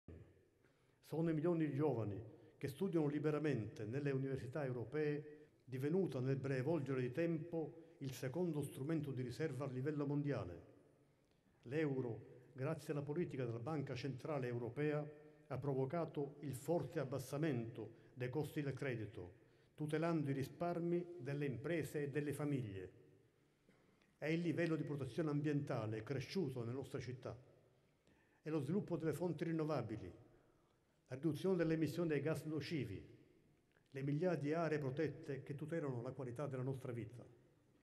Trattati di Roma, la Lega diserta le celebrazioni, ma Bossi sfida Salvini e resta alla Camera
(Agenzia Vista) - Roma, 22 Marzo 2017 - In Parlamento la celebrazione del sessantesimo anniversario della firma dei Trattati di Roma, che ricorre il 25 Marzo. Alla presenza del Presidente della Repubblica, Sergio Mattarella, si riuniscono in seduta solenne deputati, senatori, parlamentari europei eletti in Italia, rappresentanti degli organi costituzionali e alte cariche istituzionali.